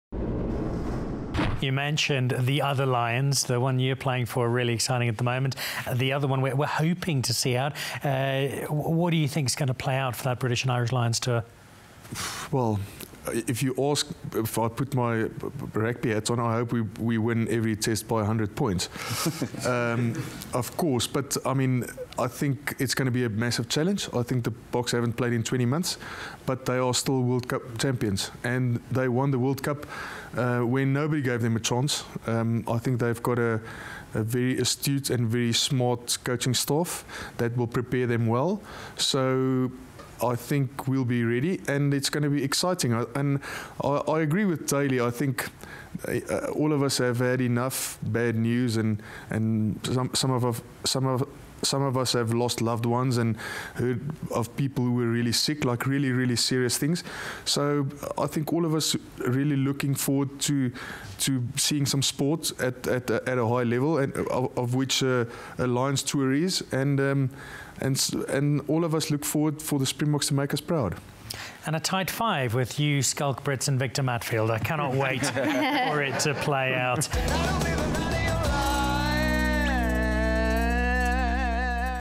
Jannie du Plessis calls the Lions tour
The front row battle was a huge factor the last time the British and Irish Lions toured South Africa, so Springbok World Cup-winner Jannie du Plessis is ideally placed to preview the upcoming series.